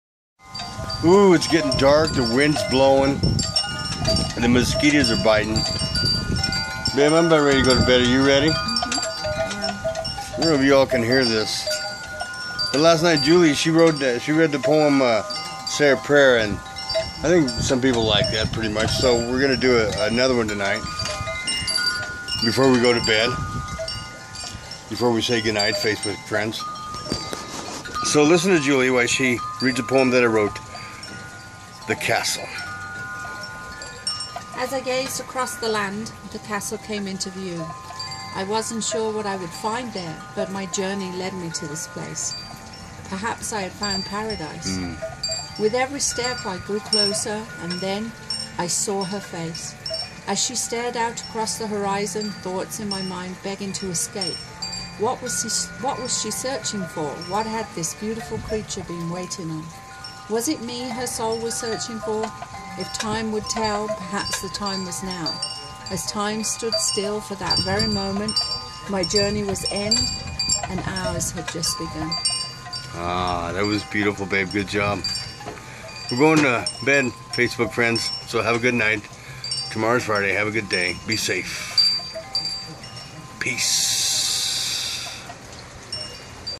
The Castle
Julie Reciting The Castle